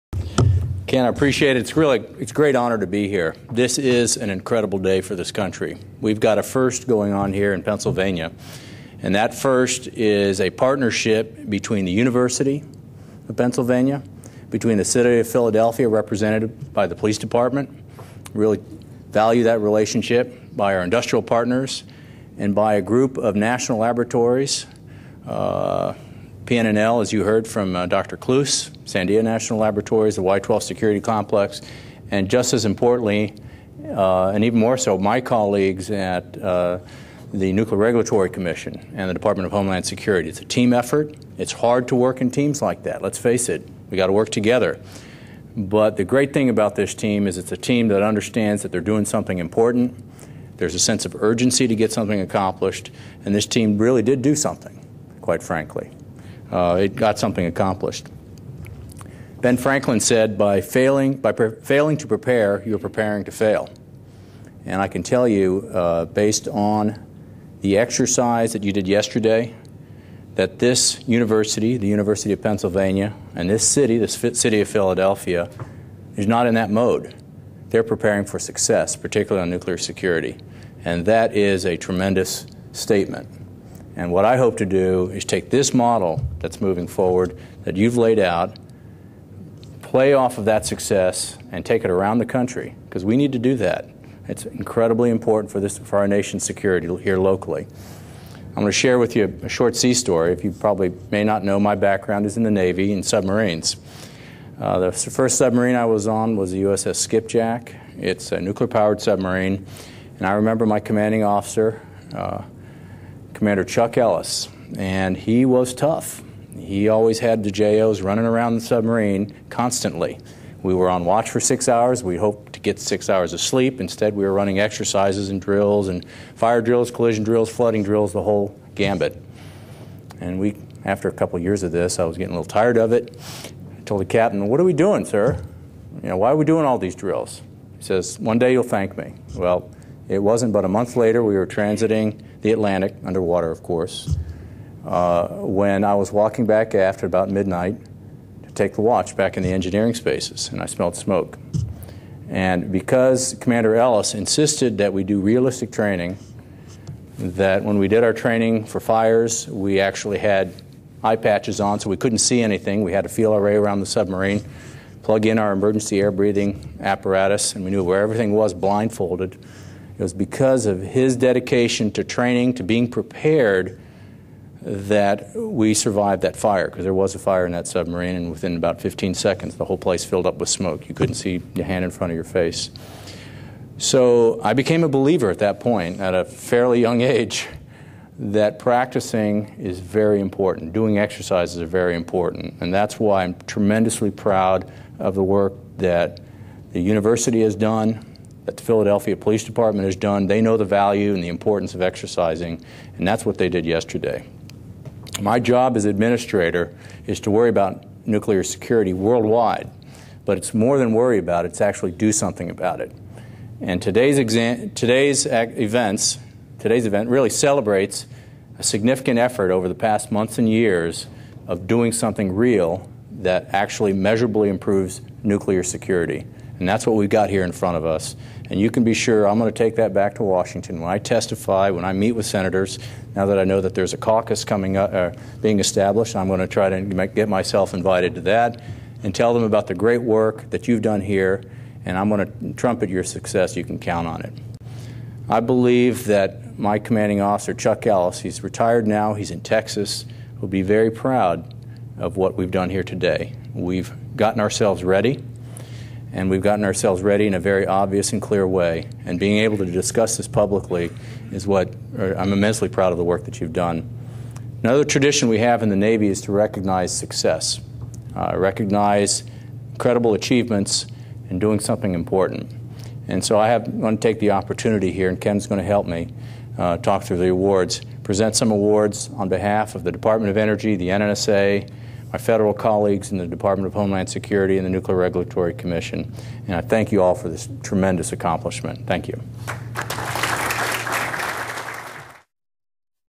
NNSA Administrator Tom D'Agostino commends UPenn, Philadelphia police
In March 2009, the National Nuclear Security Administration (NNSA) recognized the University of Pennsylvania and the city of Philadelphia for their national security leadership during a nuclear security forum.  NNSA and the Federal Bureau of Investigation hosted a table-top exercise with federal, state and local officials at the University of Pennsylvania to exercise security alarm response and crisis/consequence management skills in response to a terrorist event.